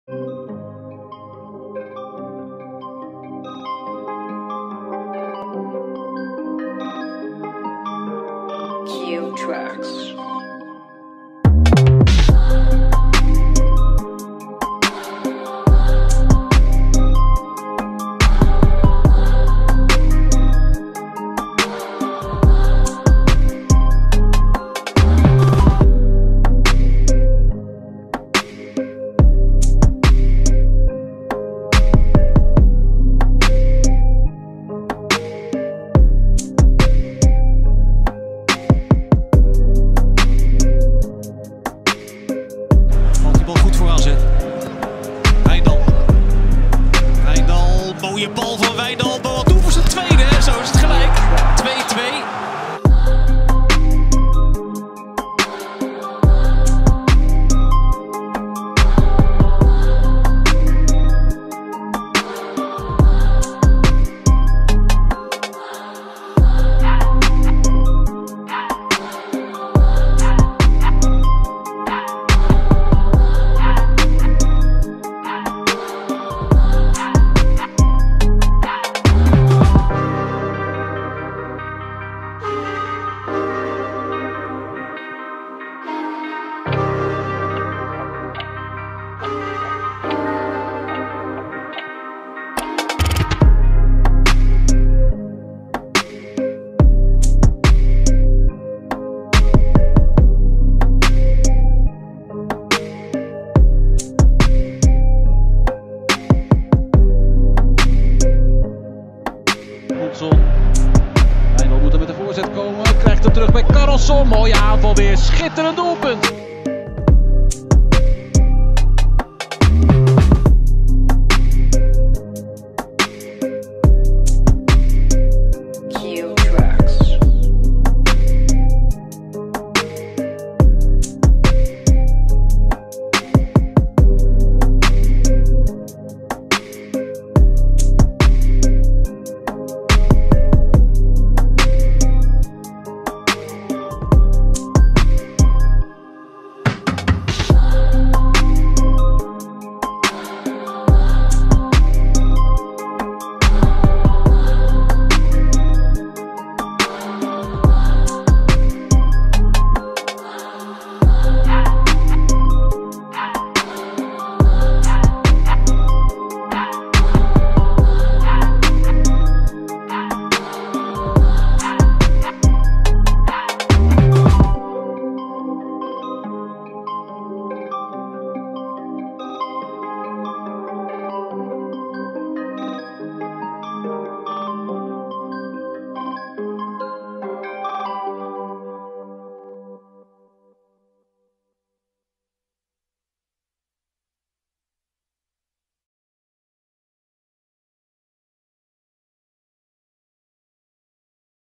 Owen Wijndal - Welcome to Ajax? - Most talented left back in the world - Skills, goals, defending -
there are many clubs interested in owen Wijndal. He is considered one of the most talented left backs in the world. Ajax and Barcelona are both doing everything they can to take over the talented player from Az Alkmaar. Together with Boadu, Stengs and Koopmeiners, he is seen as one of the best players of az Alkmaar in the 2020/21 season

Owen Wijndal best moments and highlights 2020/2021

Age: 21

nationality: Netherlands

Market value: 18 million

Current club: Az Alkmaar since 2017